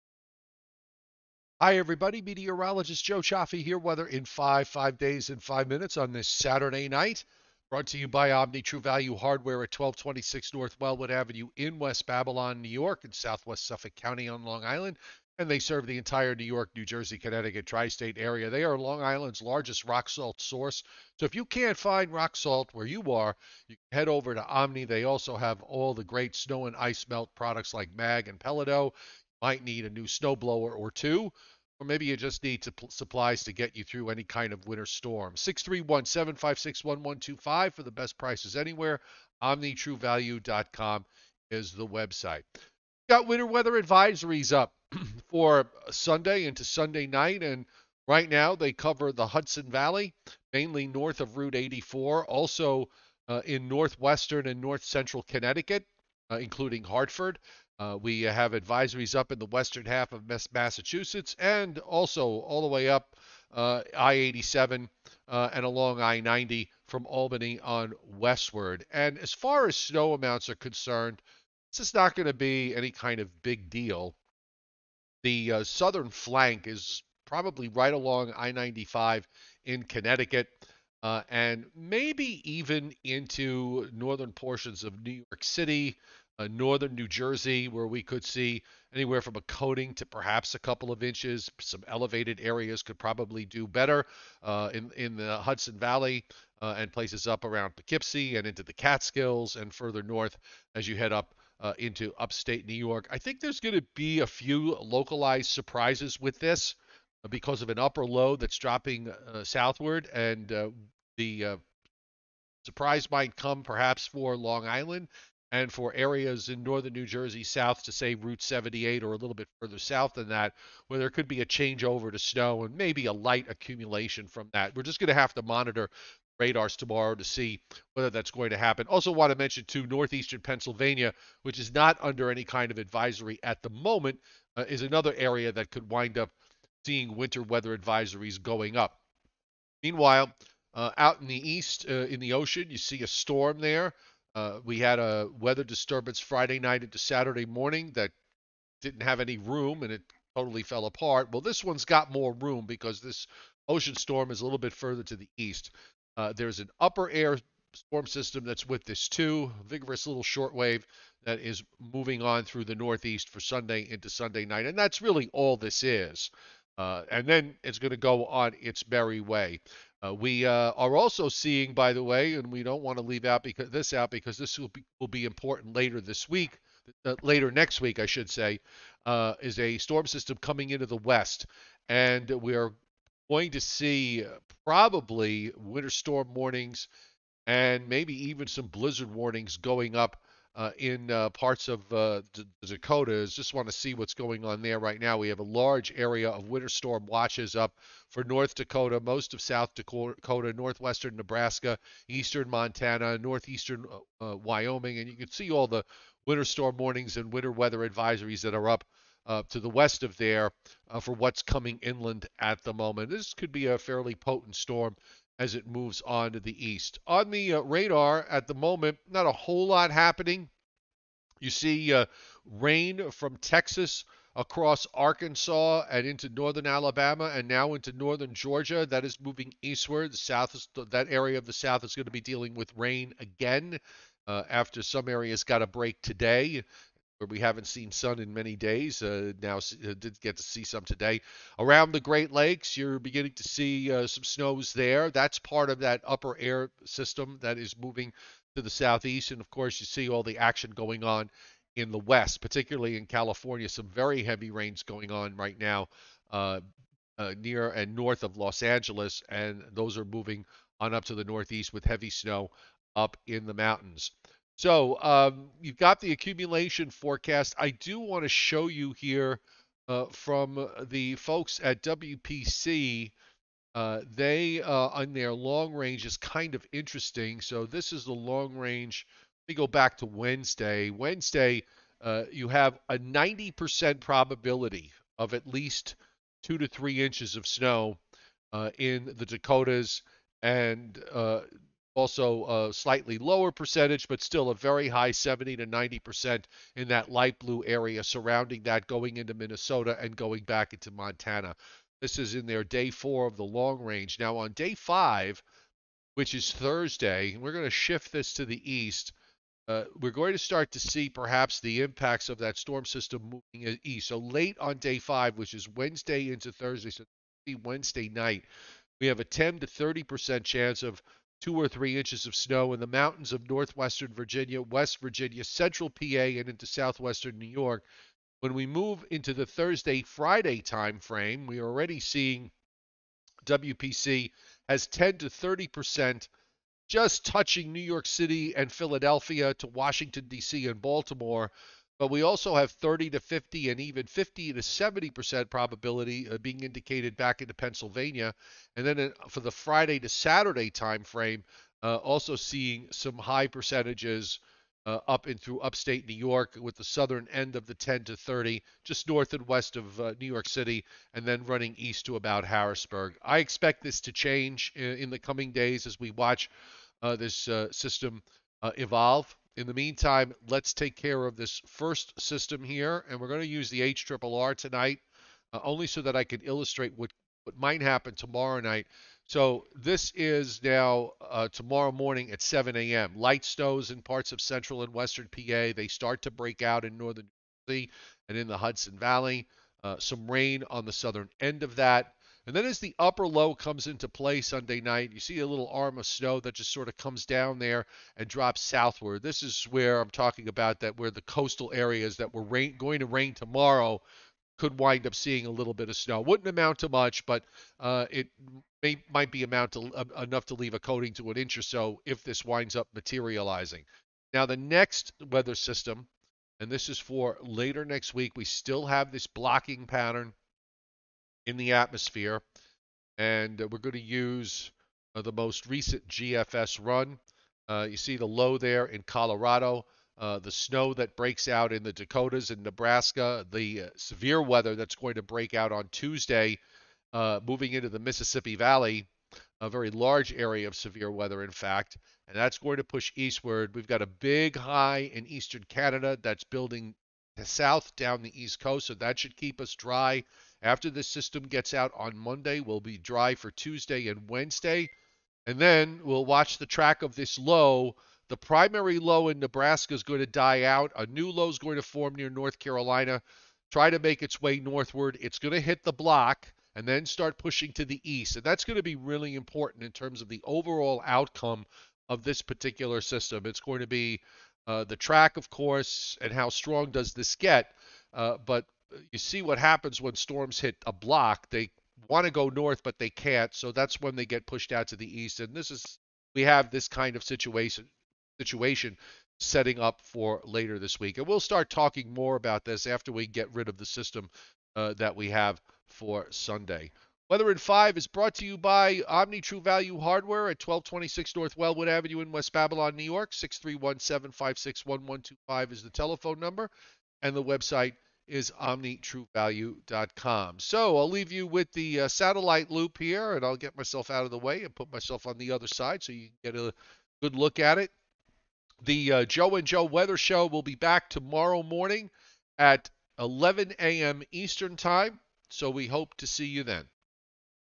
Weather in 5 Winter Weather Advisories Parts of the Northeast, Major Storm in the West Heads East
Weather in 5 a minor system is heading into parts of the Northeast Sunday with some accumulating snow and rain along the coast. Winter Weather Advisories are up inland in NY and Western New England. A strong storm in the west producing heavy rains and snow will move into the Rockies and then the Plains. We have numerous winter storm watches and winter storm warnings up from the Plains to the West Coast. This storm will bring severe weather Tuesday and Wednesday to the Mississippi Valley and then it heads to the East Coast late in the week.

Omni True Value Hardware has everything you need to get you through this winter.